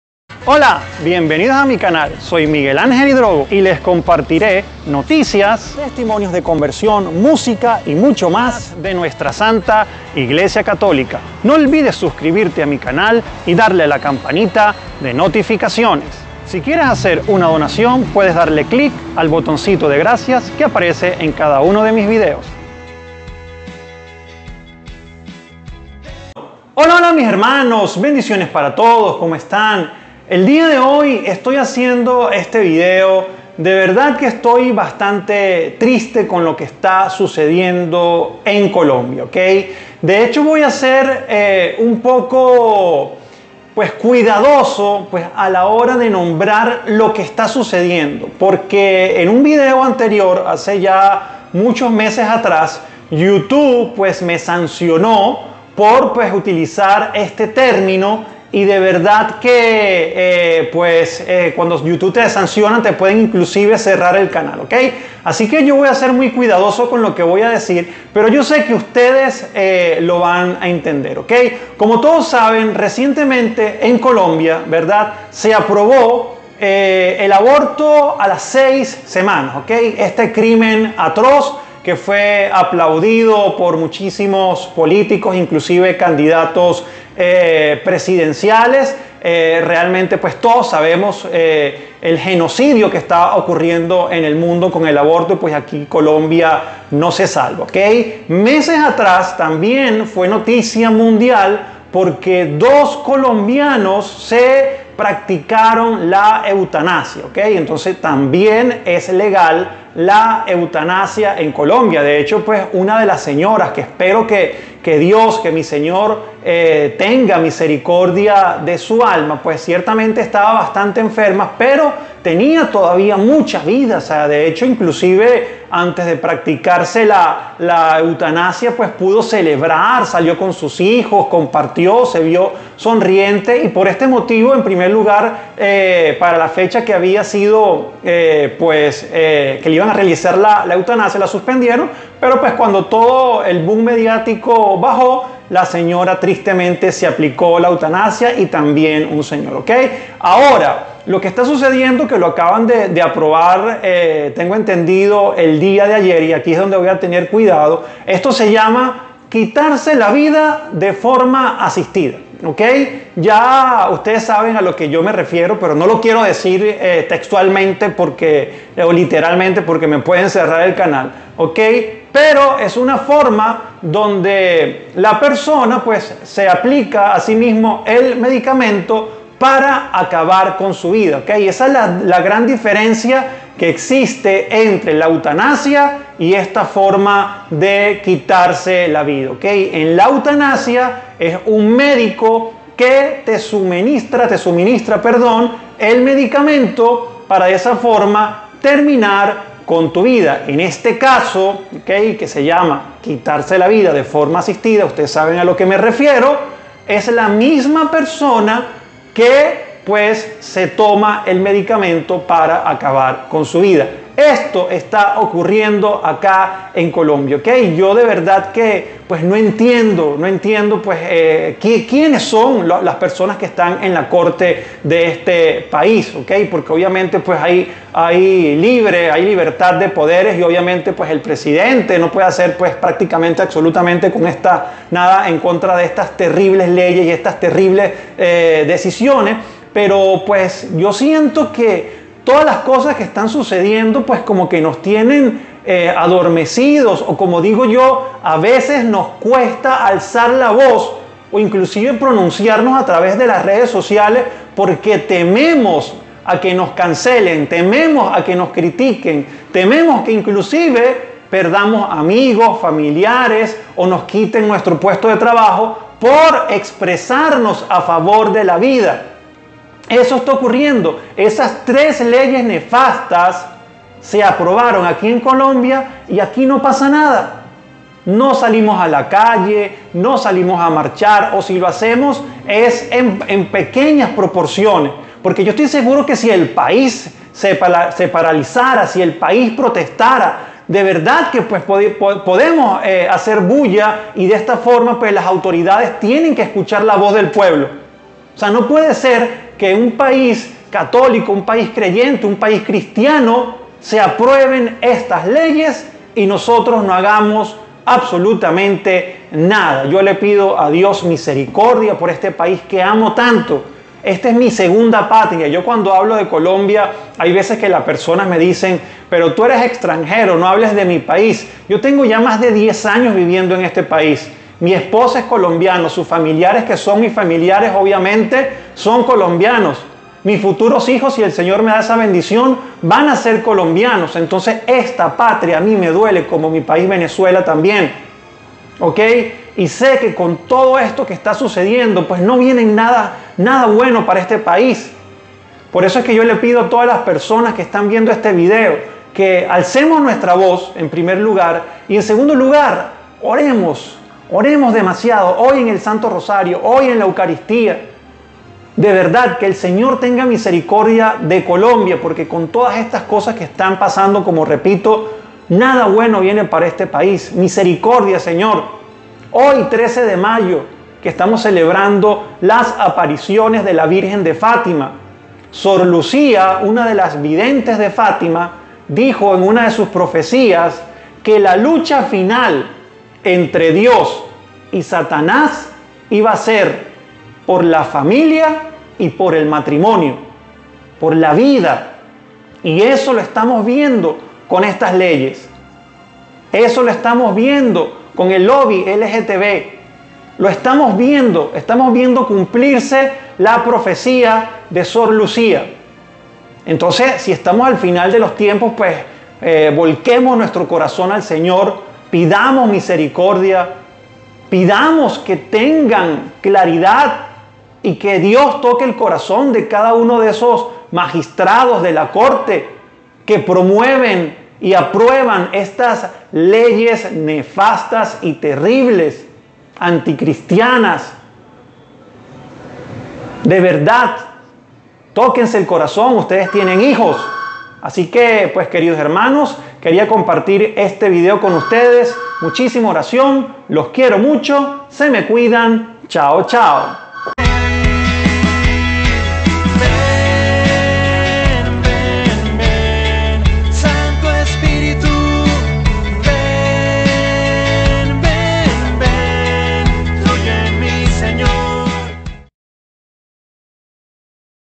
ESTO ACABA DE PASAR EN COLOMBIA 😪/SE ACABÓ EL RESPETO A LA VIDA 🤯😱
Lo que esta sucediendo en Colombia con las leyes que están aprobando es nefasto y triste, pidamos al Dios todo poderoso misericordia.